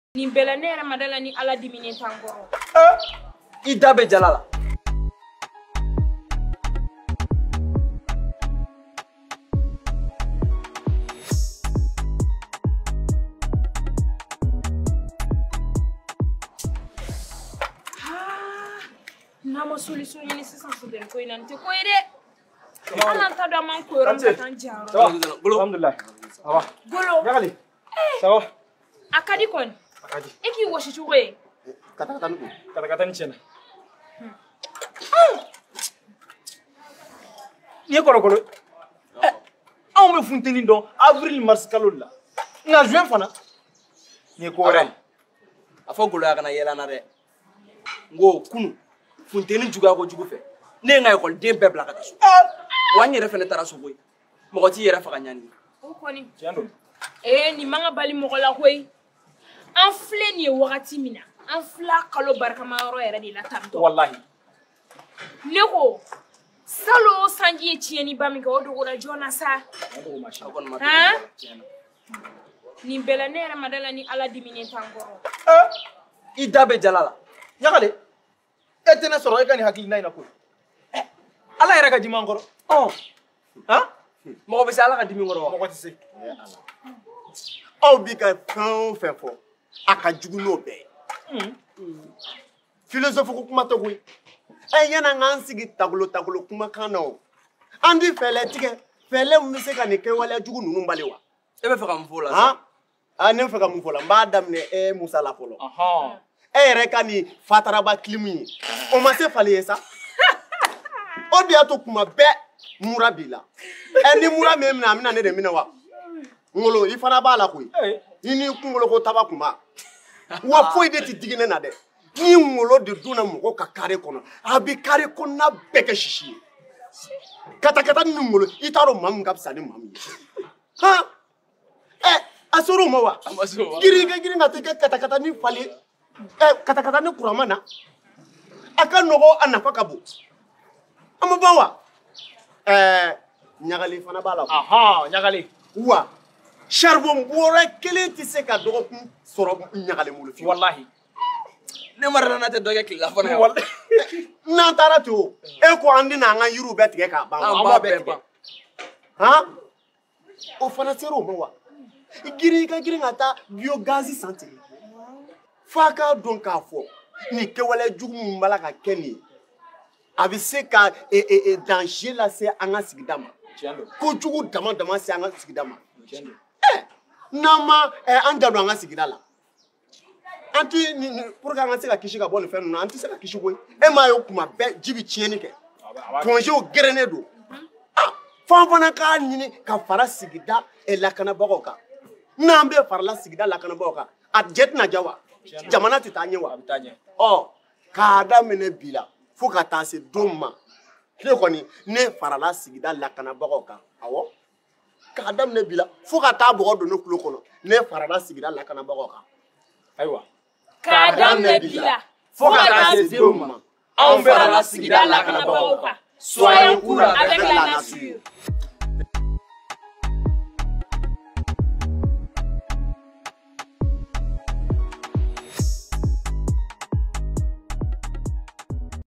Changement climatique pourquoi nous devons changer de comportement
CoolAvecLaNature, une video réalisée par Benbere pour la campagne